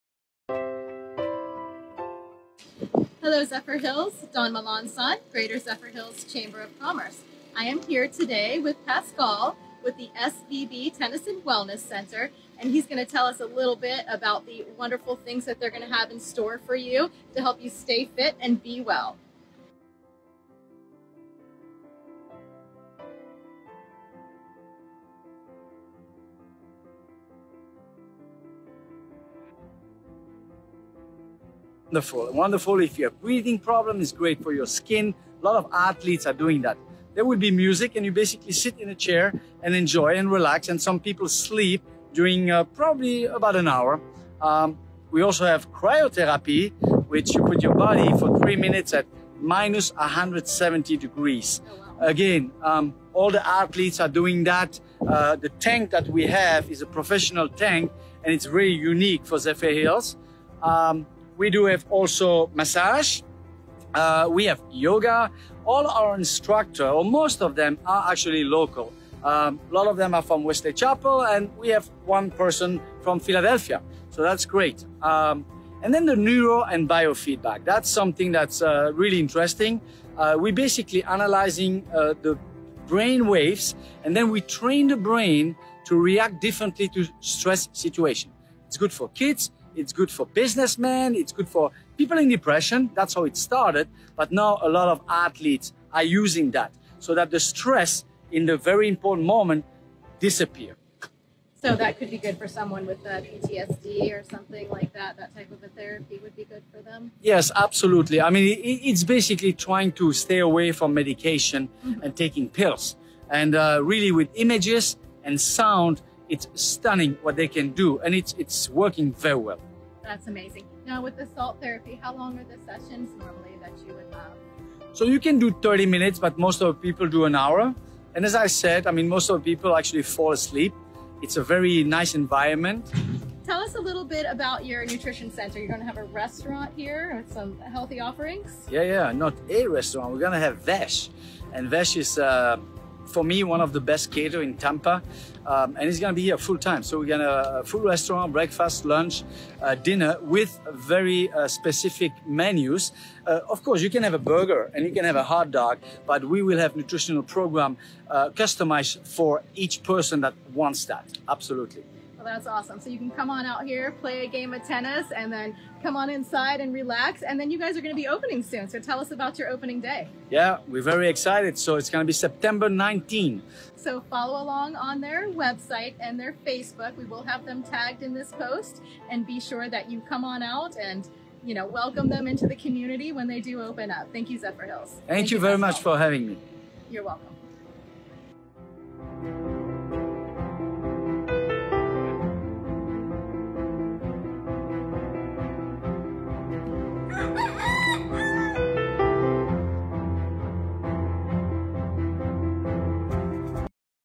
Be Well - SVB Tennis & Wellness
You will not believe what is coming to Zephyrhills! The Sarah Vande Berg Tennis & Wellness Center will be opening on September 19, 2020 (Please note: Grand Opening Date is to be announced)! SVB has graciously donated a gift voucher to include a 1 hour private lesson for a Racquet Sport of your choice valued at $125.00, which will be part of our Be Well Basket give away! Enjoy this informative video of SVB Tennis & Wellness, follow SVB on facebook, then head on down to Main Street Zephyrhills for your chance to enter in our QR Code Scavenger hunt!